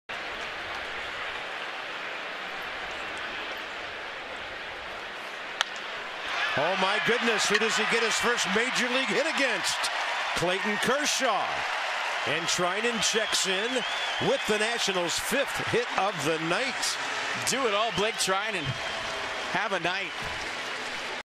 LAD@WSH: Treinen singles to notch first career hit
5/6/14: Nationals pitcher Blake Treinen lines a single off Clayton Kershaw to record his first career Major League hit